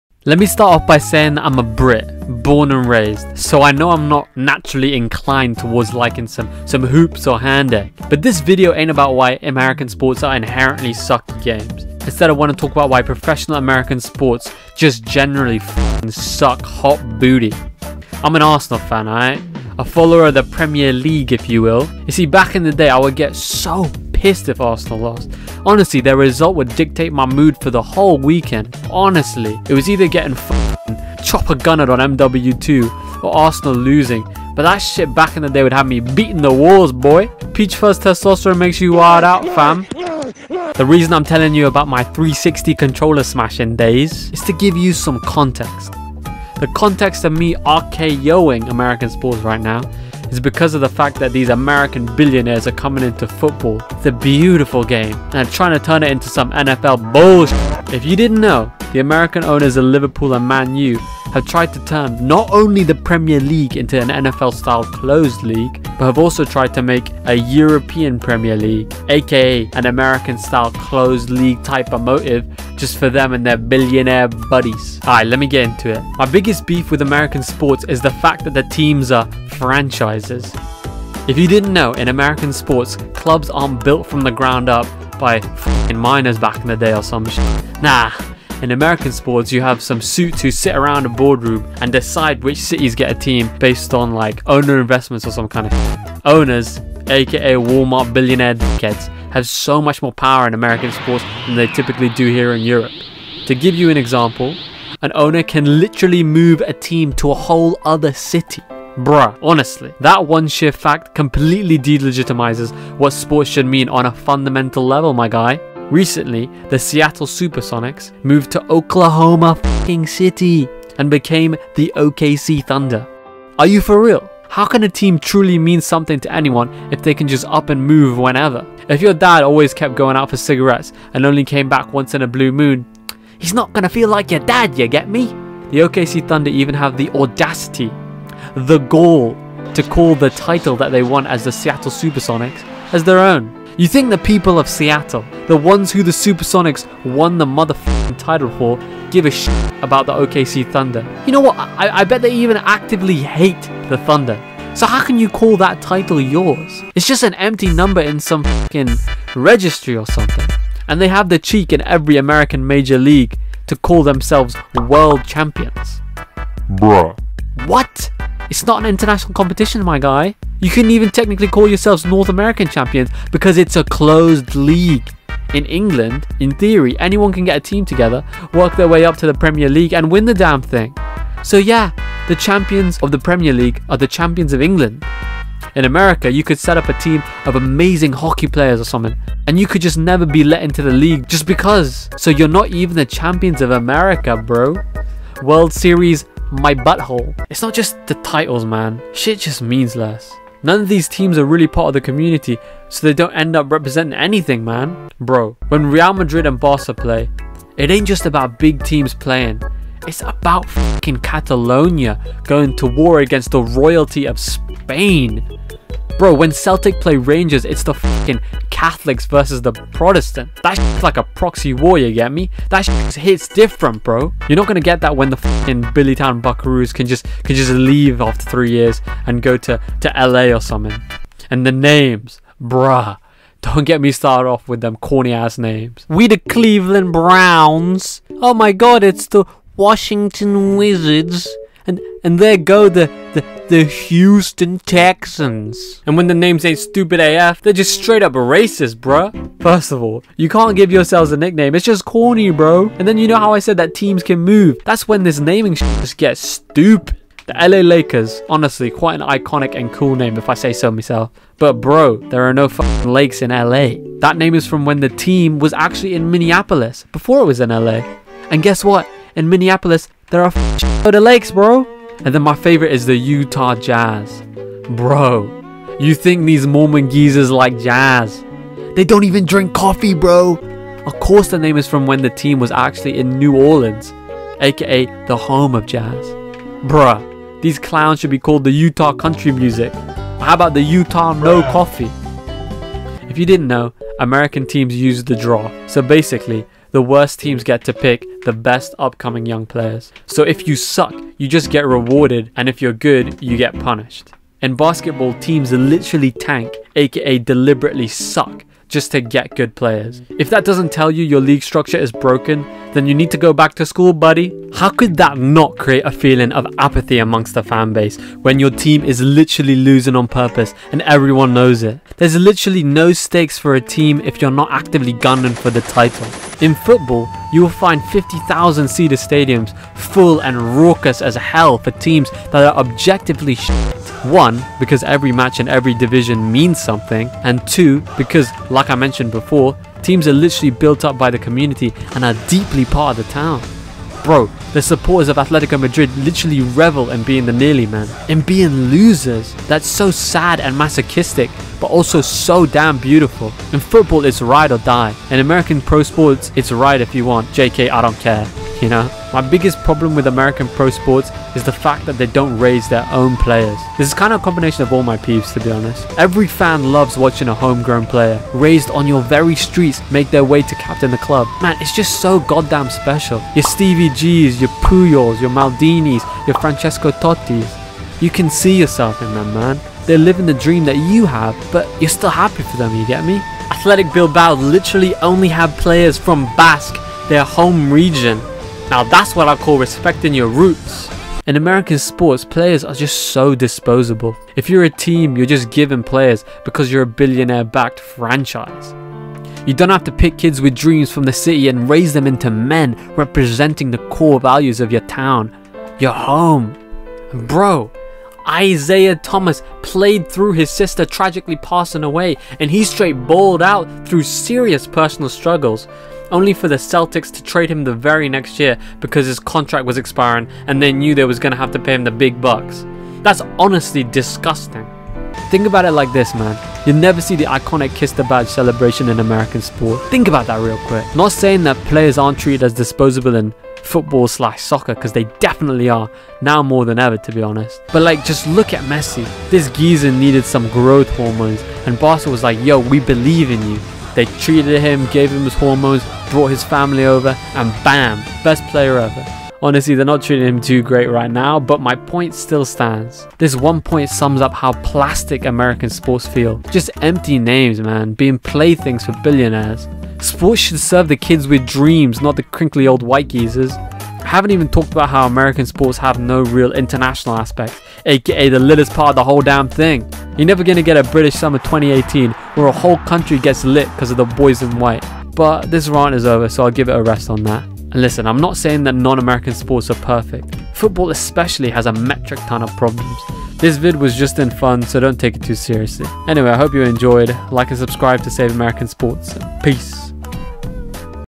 American Pro Sports Culture Is Not Compatible With European Football
As the premier league is threatened by American Investors and the looming threat of the European Premier League, ya boy bang lays out to you why the NFL, NBA, NHL, MLS, MLB and basically the very core of American professional sports suck. Sorry to be that guy but American sports are the superbowl of corporate control and vested financial interests versus passion.